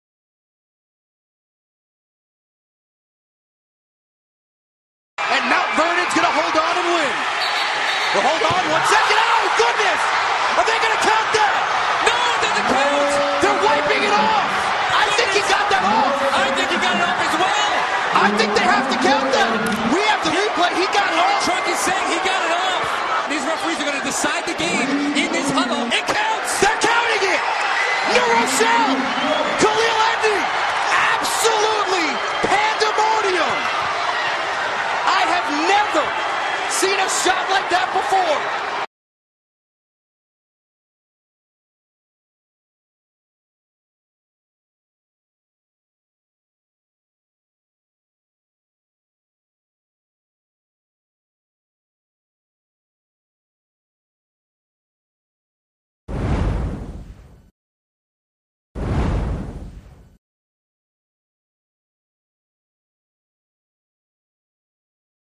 Raw: HS Hoops Player Sinks 55-Foot Buzzer Beater
This buzzer beater is going to be tough to beat just three days into March. Khalil Edney grabbed a loose ball and made a 55-footer as time expired to give New Rochelle a 61-60 win over rival Mt. Vernon in a New York sectional final on Sunday. (March 4)